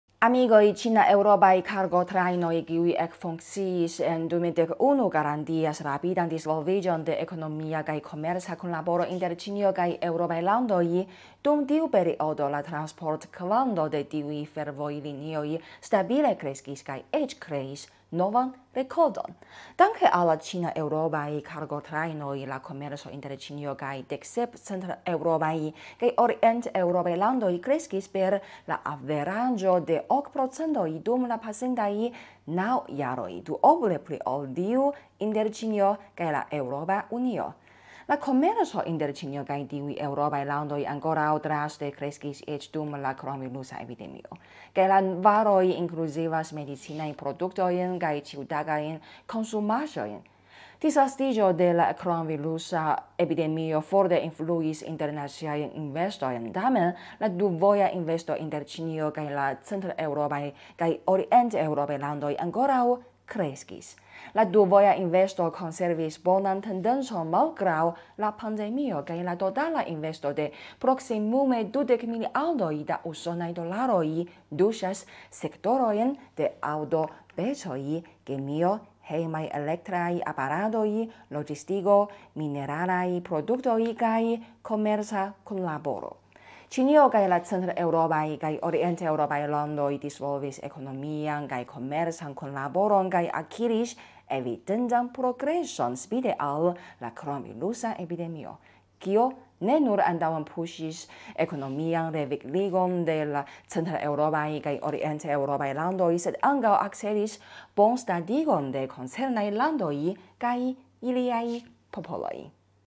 Komerco inter Ĉinio kaj centr- kaj orient-eŭropaj landoj kreskis malgraŭ pandemio
Amikoj, ĉina-eŭropaj kargotrajnoj, kiuj ekfunkciis en 2011, garantias rapidan disvolviĝon de ekonomia kaj komerca kunlaboro inter Ĉinio kaj eŭropaj landoj. Dum tiu periodo, la transportkvanto de tiuj fervojlinioj stabile kreskis kaj eĉ kreis novan rekordon.
Danke al la ĉina-eŭropaj kargotrajnoj, la komerco inter Ĉinio kaj 17 centreŭropaj kaj orienteŭropaj landoj kreskis per la averaĝo de 8% dum la pasintaj naŭ jaroj, duoble pli ol tiu inter Ĉinio kaj la EU. La komerco inter Ĉinio kaj tiuj eŭropaj landoj ankoraŭ draste kreskis eĉ dum la kronvirusa epidemio, kaj la varoj inkluzivas medicinajn produktojn kaj ĉiutagajn konsumaĵojn.
Disvastiĝo de la kronvirusa epidemio forte influis internaciajn investojn, tamen la duvoja investo inter Ĉinio kaj la centreŭropaj kaj orienteŭropaj landoj ankoraŭ kreskis. La duvoja investo konservis bonan tendencon malgraŭ la pandemio, kaj la totala investo de proksimume 20 miliardoj da usonaj dolaroj tuŝas sektorojn de aŭtopecoj, kemio, hejmaj elektraj aparatoj, loĝistiko, mineralaj produktoj kaj komerca kunlaboro.
Ĉinio kaj la centreŭropaj kaj orienteŭropaj landoj disvolvis ekonomian kaj komercan kunlaboron kaj akiris evidentan progreson spite al la kronvirusa epidemio, kio ne nur antaŭenpuis ekonomian revigligon de la centreŭropaj kaj orienteŭropaj landoj, sed ankaŭ akcelis bonstatigon de koncernaj landoj kaj iliaj popoloj.